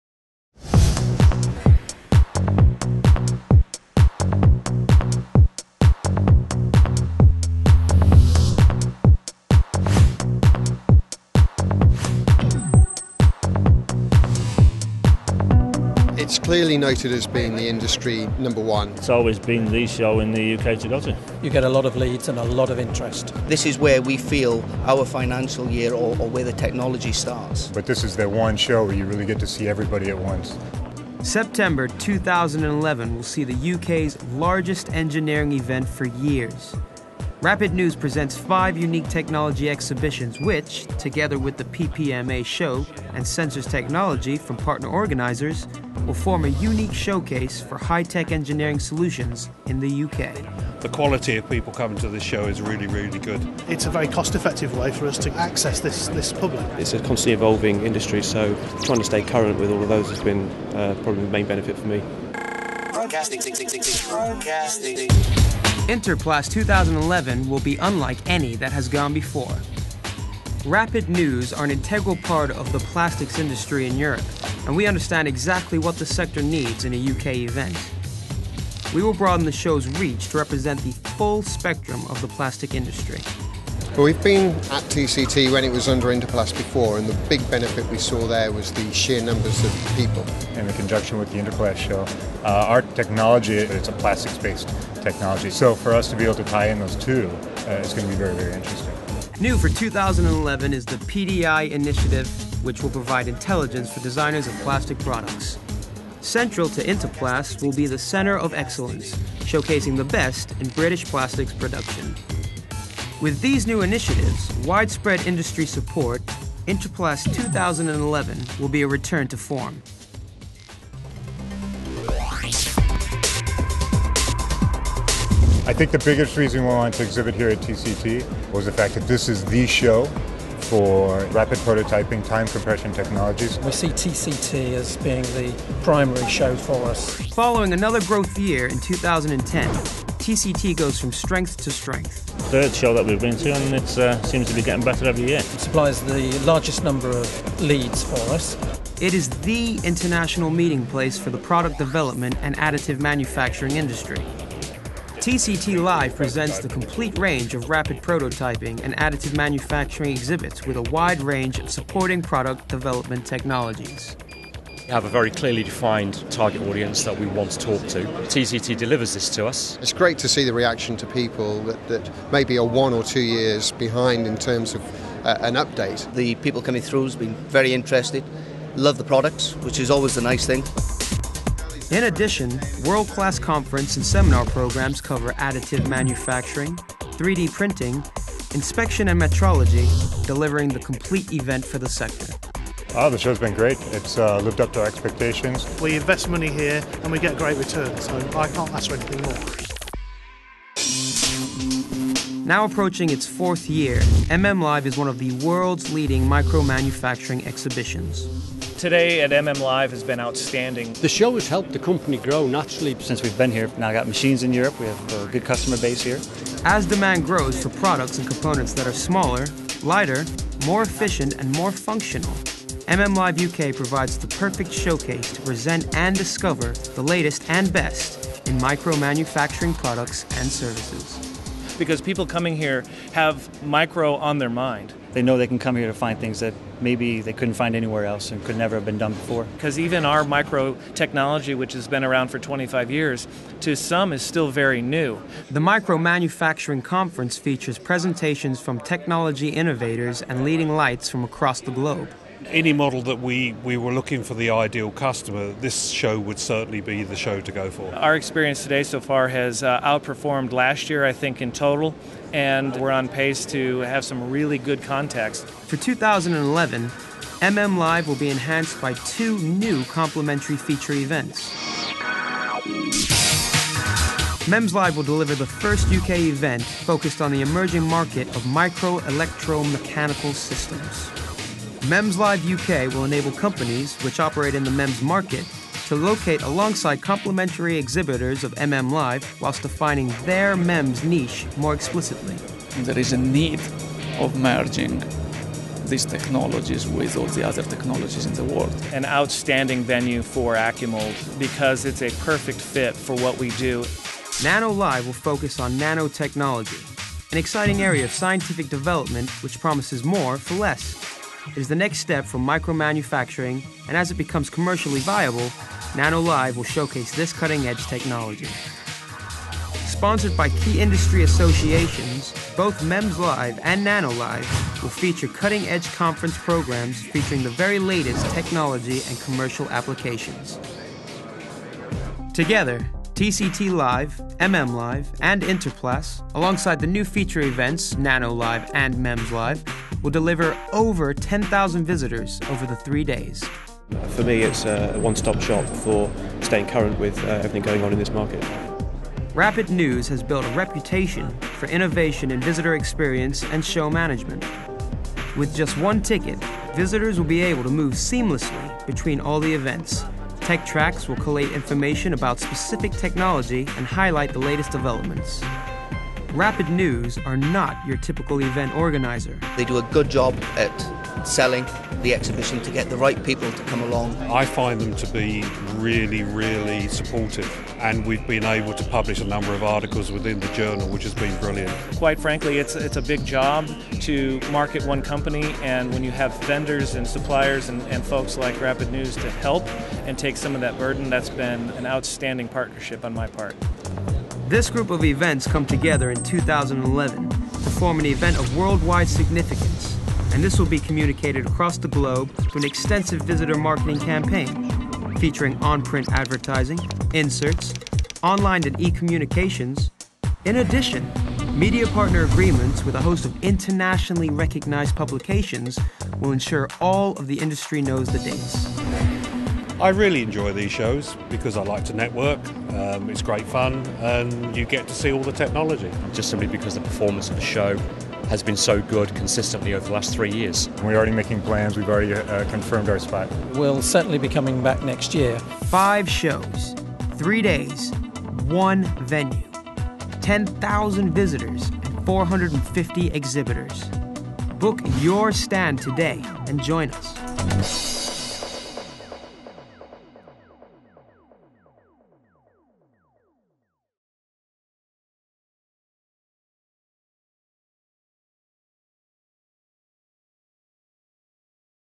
The Manufacturing Event 2011
Rapid News bring together 5 cutting edge technology events at the NEC, Birmingham on September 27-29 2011. TCT Live, Interplas, MM Live, MEMS Live and NANO Live will present over 400 exhibitors displaying the latest in engineering technology.